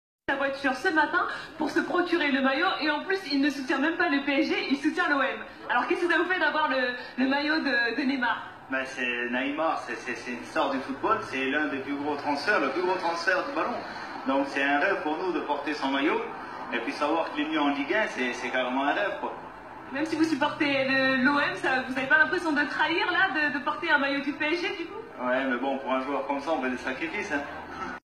LE PLUS GROS FOOTIX DE TOUTE L'HISTOIRE DU FOOTBALL !!!
IL A ACHETE LE MAILLOT DU PSG FLOQUÉ NEYMAR ALORS QU'IL SUPPORTE L'OM